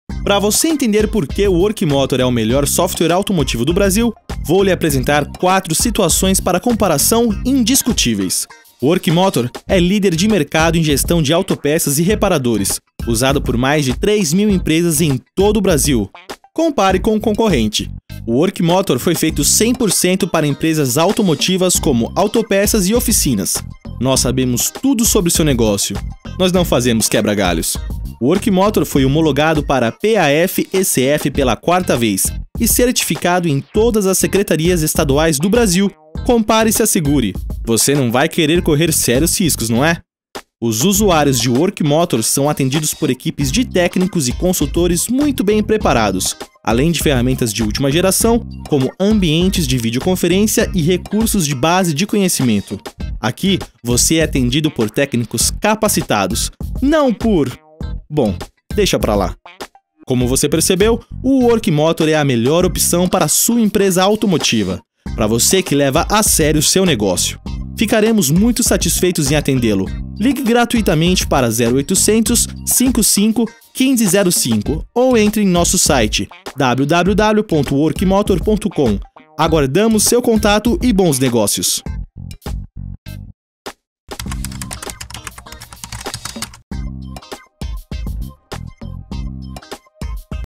WorkMotor compare e saiba pq é o melhor software para autopeças e oficinas
VIdeos comparando 4 tópicos que mostra porque o WorkMotor é o melhor software automotivo do Brasil